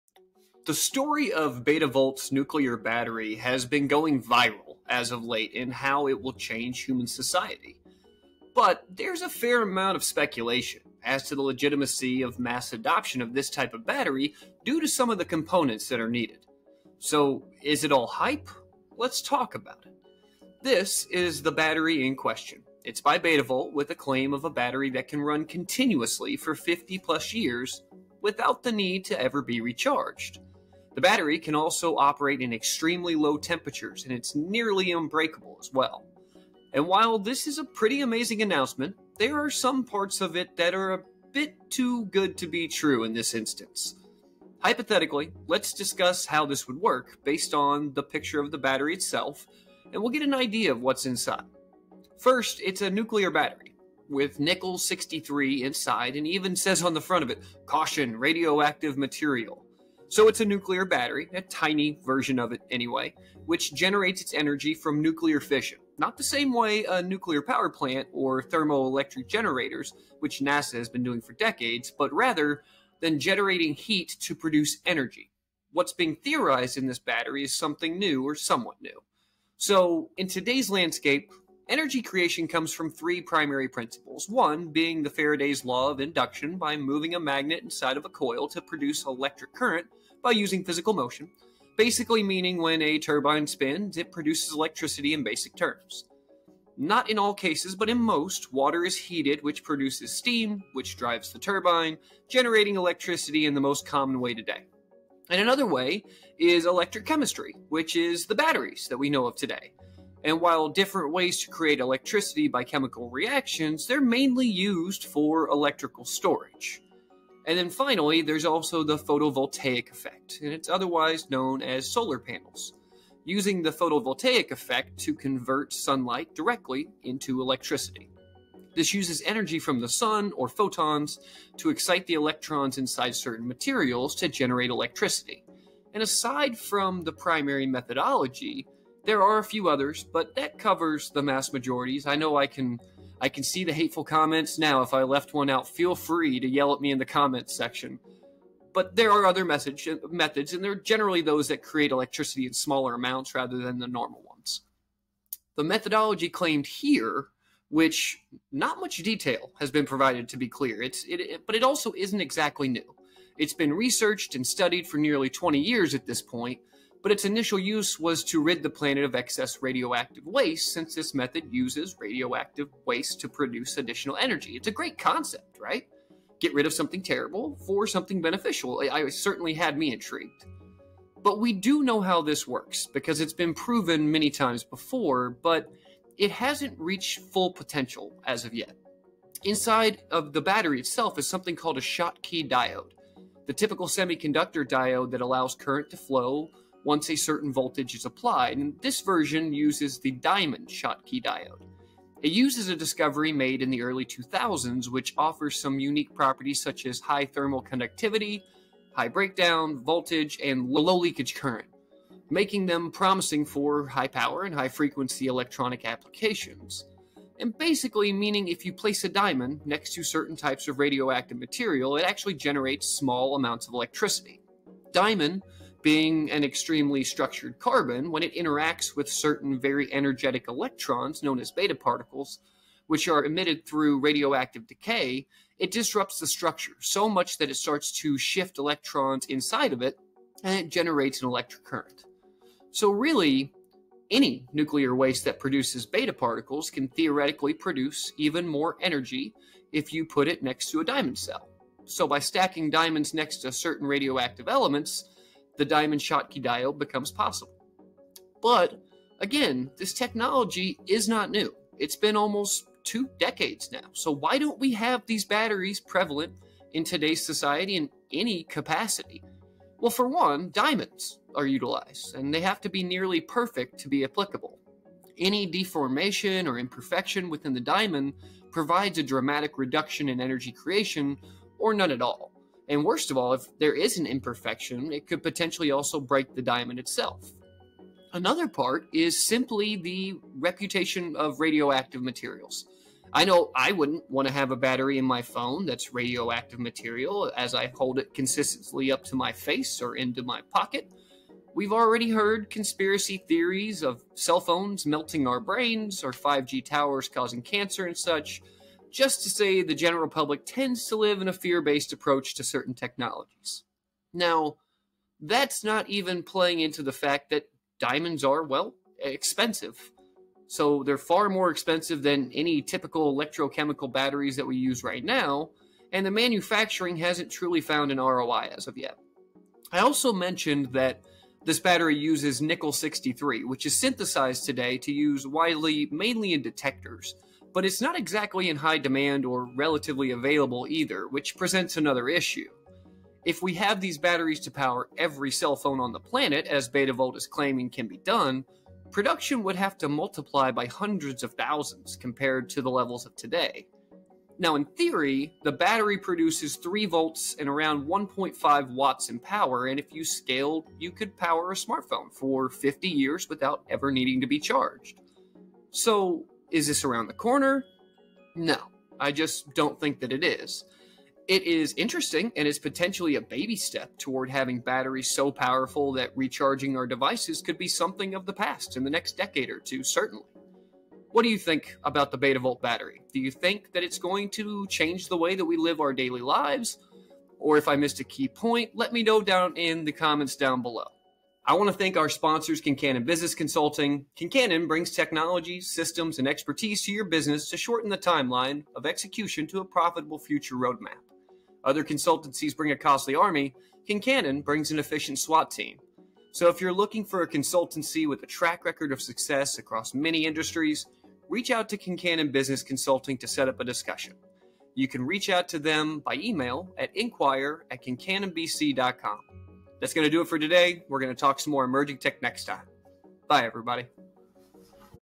How Betavolt's Nuclear Battery (Somewhat) Surprised Scientists with Its 50 Year Power Lifespan
Explore the remarkable narrative of Betavolt's Nuclear Battery in this episode of Emerging Technologies in Business. With its claim of a 50-year power lifespan, this innovation has captured global intrigue. But amidst the excitement, questions linger over its feasibility for mass adoption due to complex components.

Dive into the battery's core—Nickel 63 and Diamond Schottky diode—and its novel energy generation from radioactive decay. Yet, challenges abound, from diamond perfection demands to public concerns about radioactive materials.

Join the conversation on Betavolt's Nuclear Battery—will it redefine energy sustainability, or is it merely a tantalizing concept?